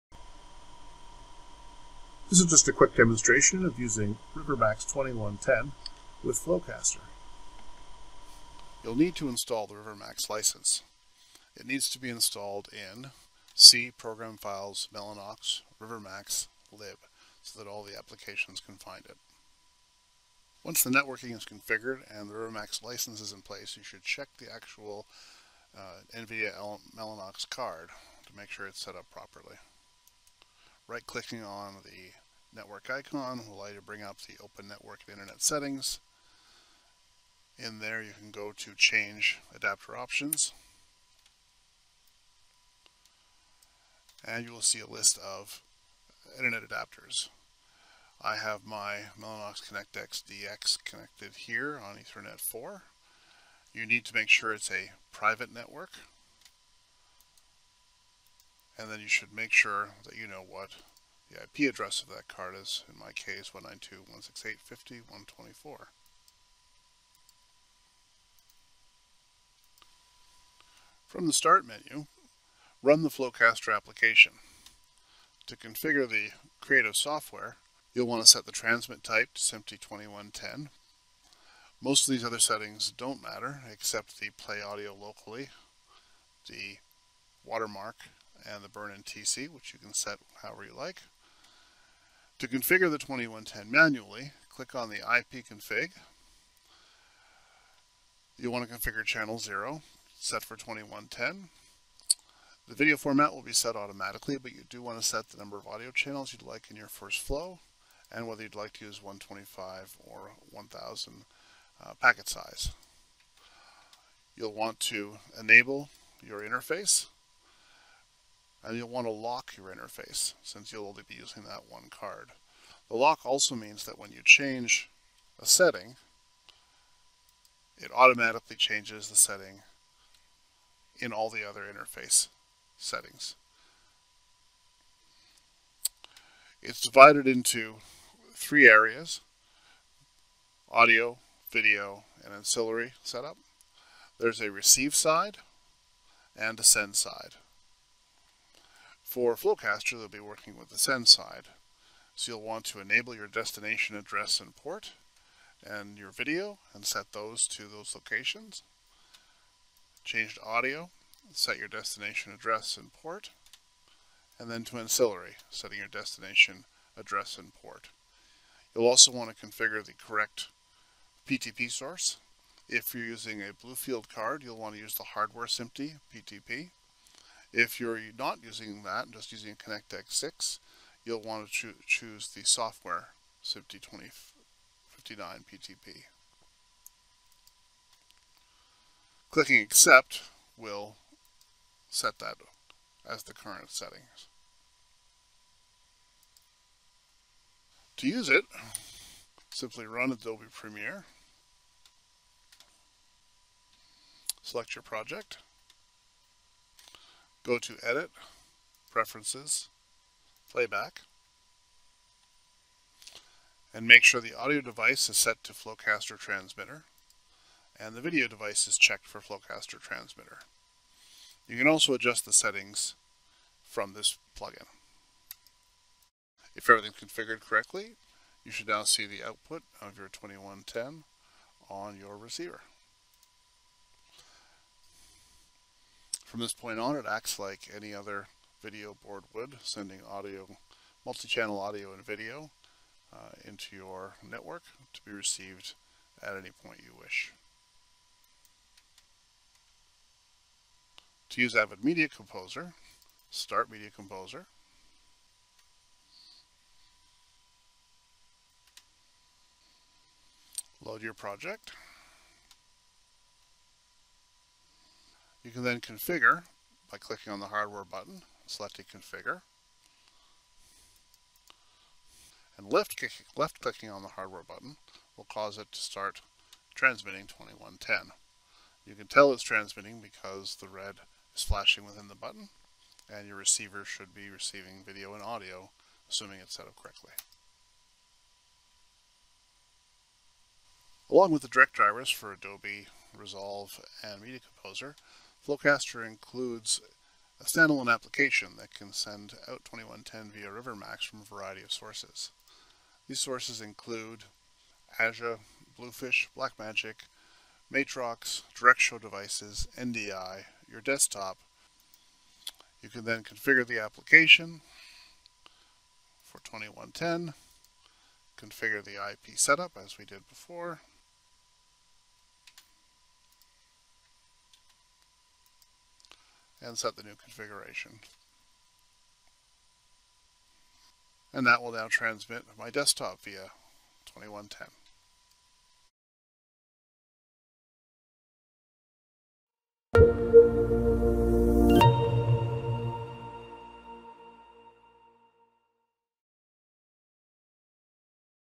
Rivermax FlowCaster manual setup
A quick demonstration of a manual setup of FlowCaster using NVIDIA/Mellanox Rivermax for SMPTE 2110